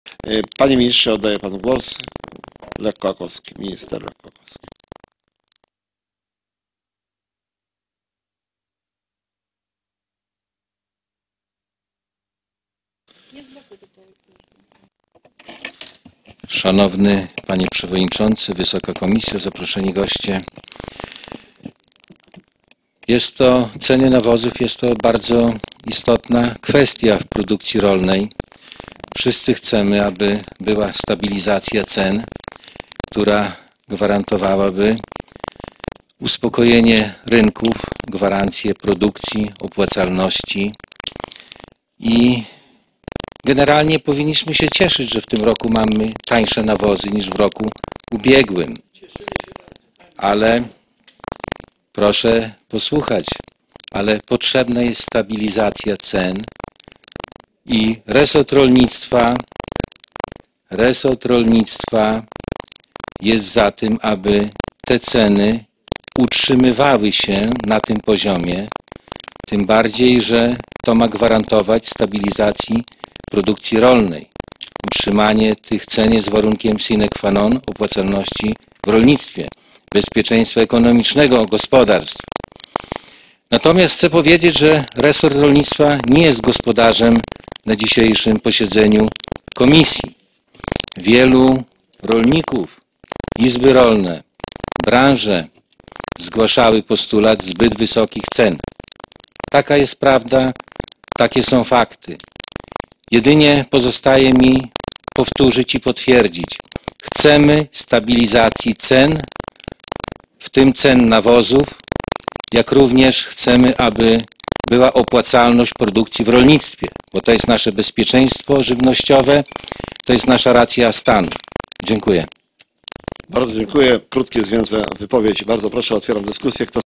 Lech Kołakowski - generalnie powinniśmy się cieszyć że ... , Sytuacja na rynku nawozów - część 2
Lech Kołakowski  sekretarz stanu w Ministerstwie Rolnictwa i Rozwoju Wsi

Komisja Rolnictwa i Rozwoju Wsi
I. Rozpatrzenie projektu dezyderatu Komisji Rolnictwa i Rozwoju Wsi do Wiceprezesa Rady Ministrów, Ministra Rolnictwa i Rozwoju Wsi na temat zastosowania bionawozów w polskim rolnictwie oraz perspektyw ich wdrażania.
II. Rozpatrzenie Informacji na temat sytuacji na rynku nawozów sztucznych
- przedstawiają: Minister Aktywów Państwowych oraz Minister Rolnictwa i Rozwoju Wsi.

Leszek Galemba (zastępca przewodniczącego) - Posiedzenie nr 171 dnia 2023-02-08